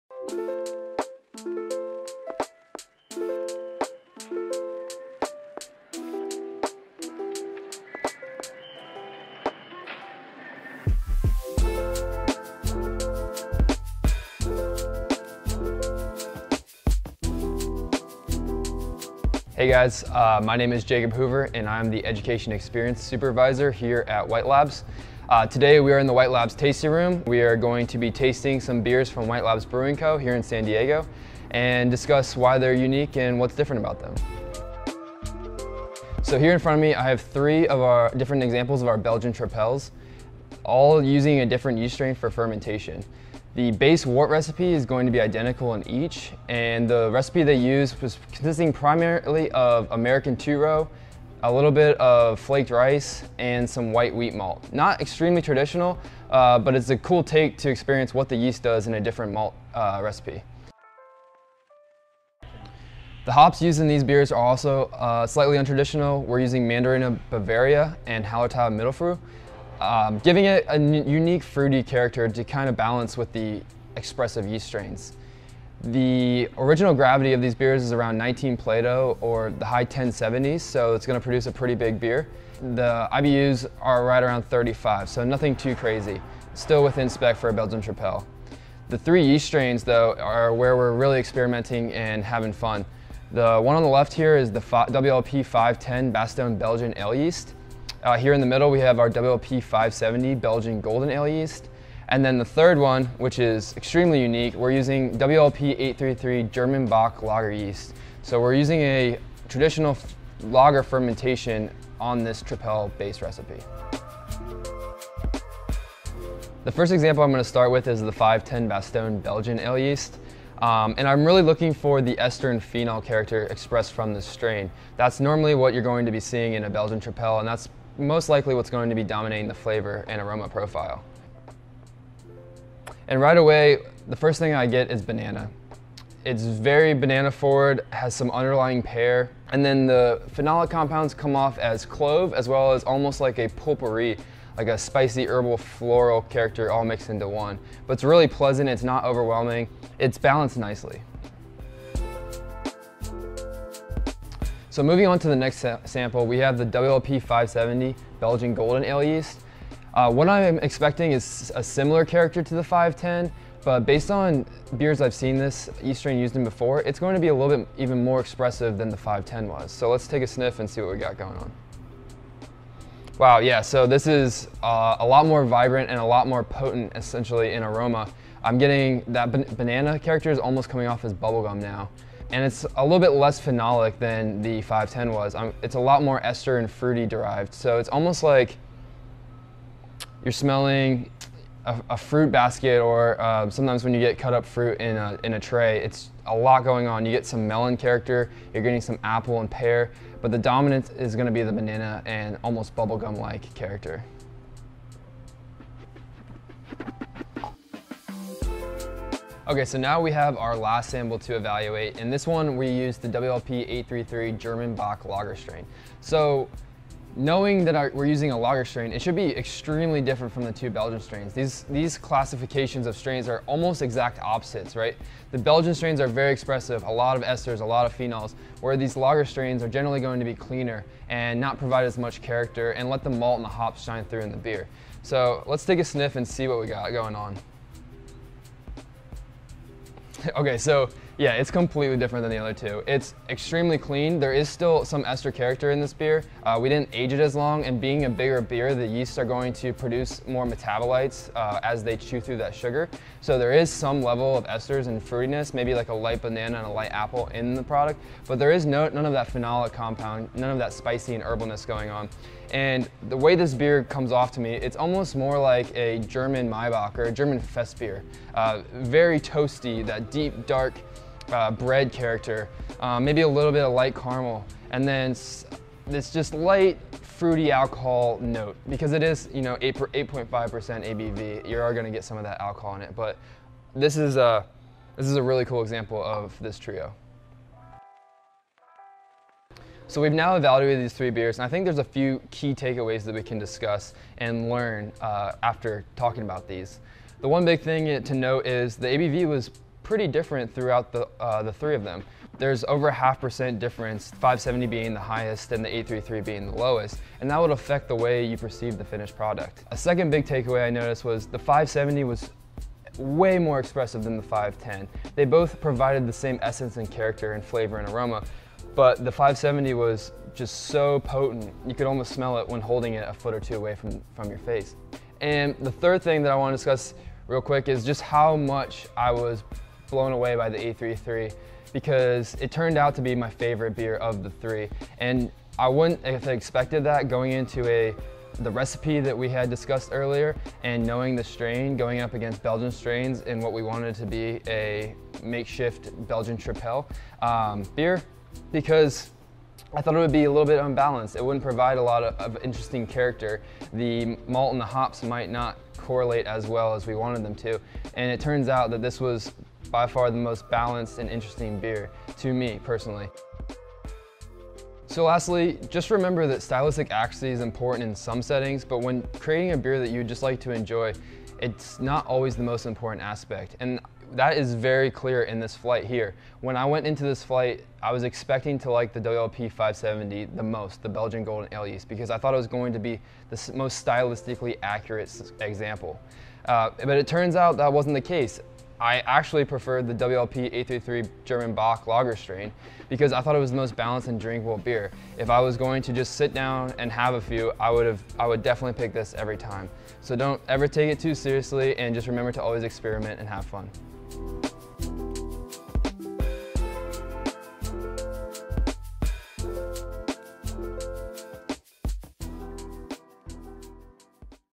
The best yeast for a Belgian Tripel - tips from the pitch
Comparing 3 different White Labs yeasts for Belgian Tripel